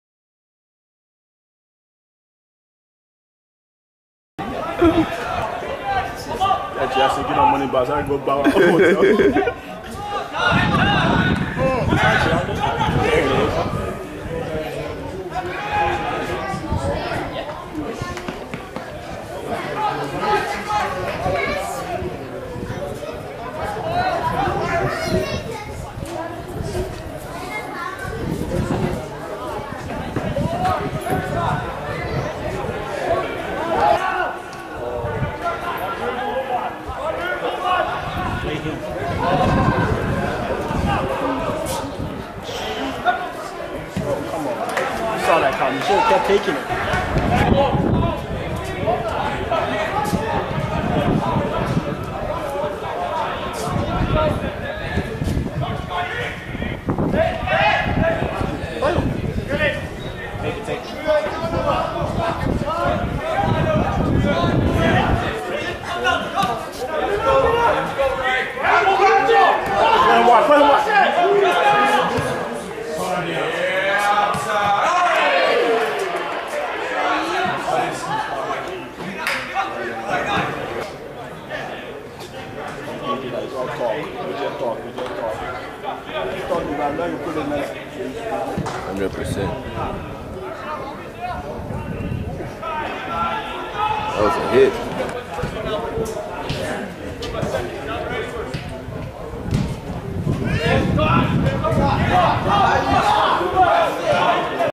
Scott Angevine Match Highlights vs. FC Roskilde (Denmark NordicBet LIGA) - 07/2017
Scott Angevine match highlights in a friendly between FC Roskilde (Denmark 1st Division) and Bridges FC (USA)